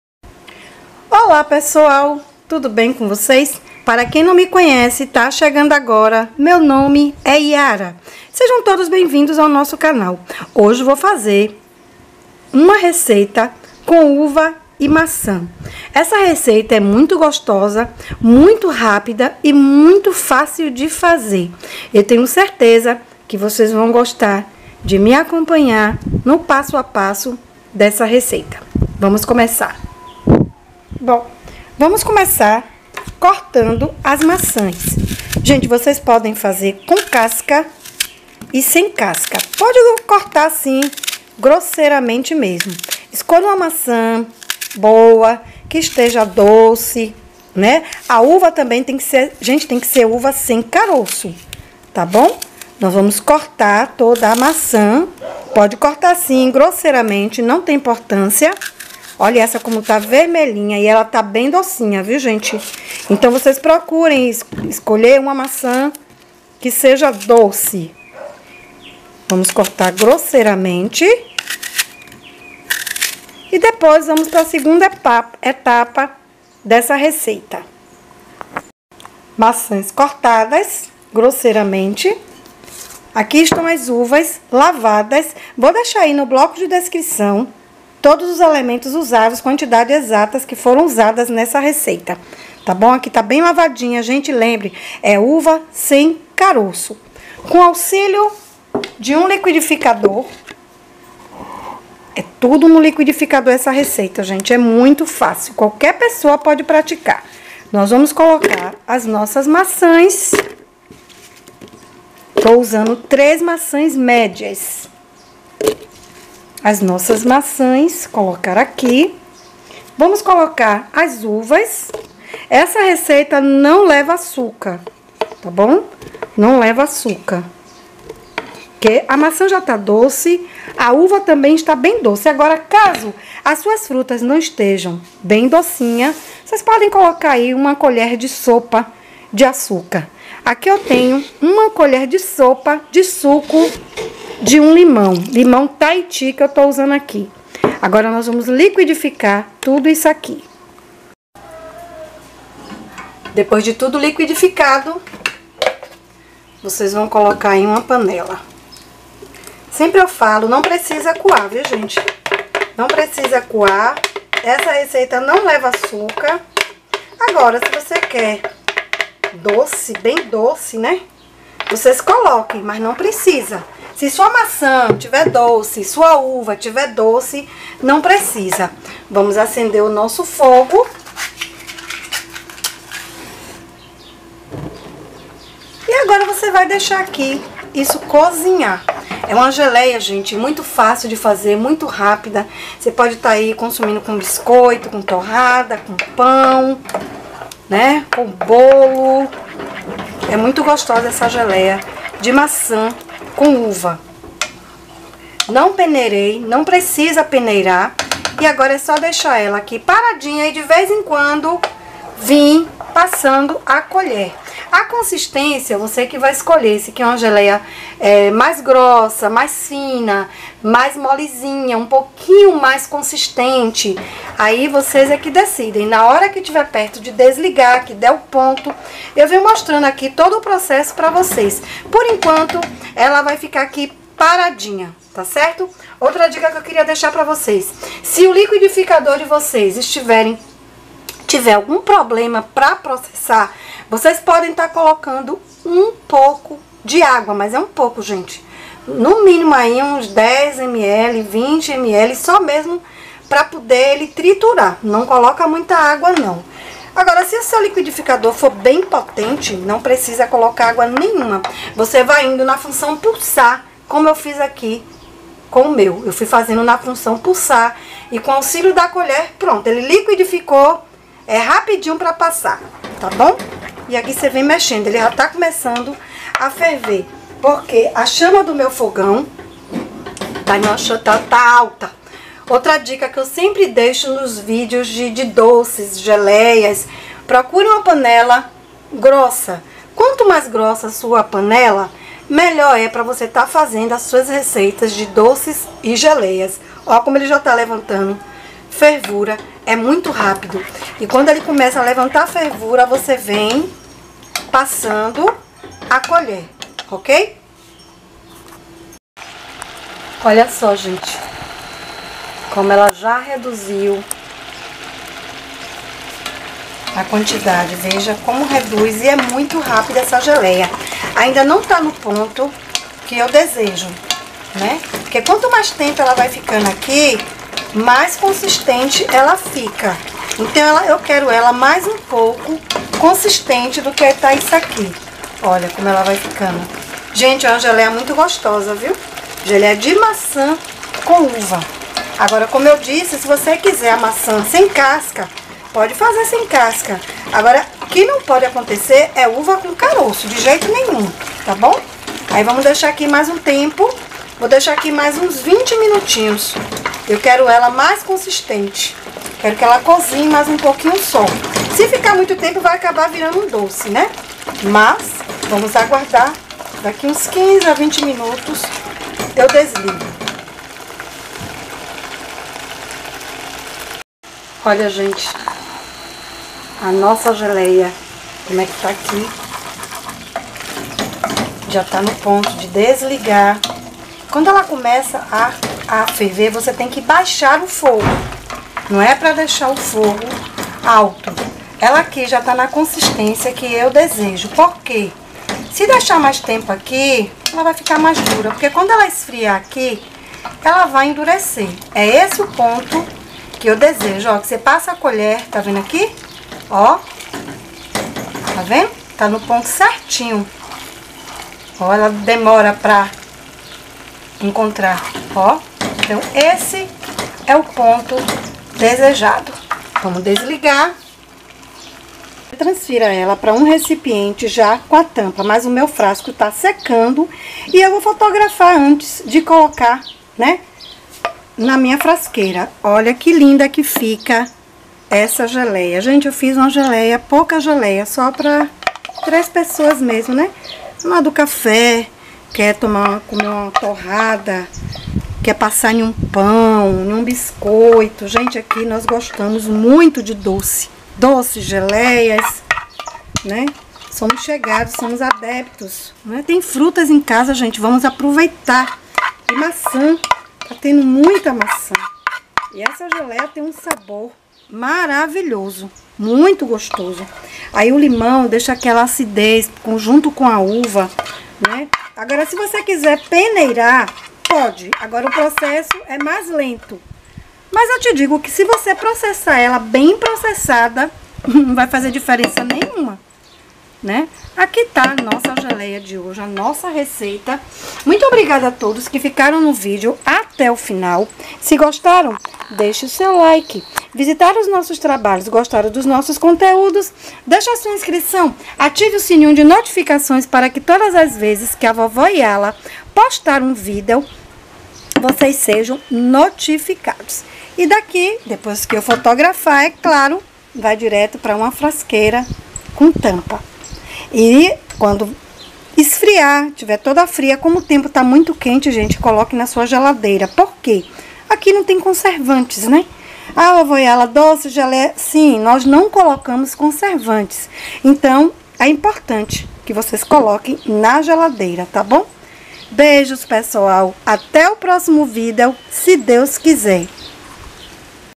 GELEIA SAUDÁVEL COM APENAS TRÊS INGREDIENTES MENOS CALÓRICA/Receita de geleia sem açúcar
Olá pessoal!
Compartilho com vocês uma receita muito fácil e rápida de fazer, além de tudo só leva três ingredientes.
Essa receita de geleia é muito gostosa, tem uma leveza ao seu sabor. Sem conservantes e sem açúcar.
Espero muito que gostem, obrigada!

Ingredientes:
três maçãs grandes
400 g de uvas sem caroços
20 ml de suco de limão

MODO DE PREPARO ACOMPANHEM AO VÍDEO, OBRIGADA!